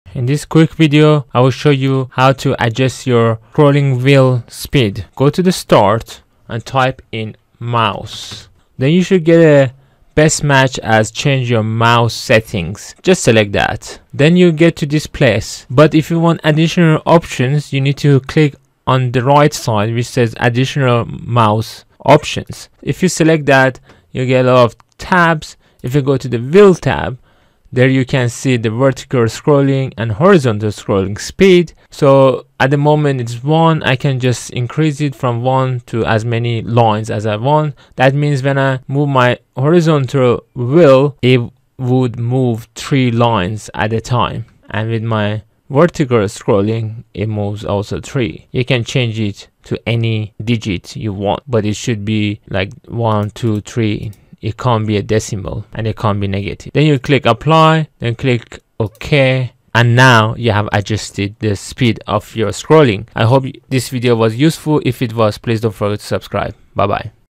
How to Customize Your Mouse Scroll Speed in Windows 10
In this short video, I'll show you how to change your mouse scroll speed in Windows 10. In this way, you could change both the vertical scroll wheel speed as well the horizontal wheel (thumb wheel)

This video is for information only. The creator of this video/channel will not be held responsible for any damage or loss caused.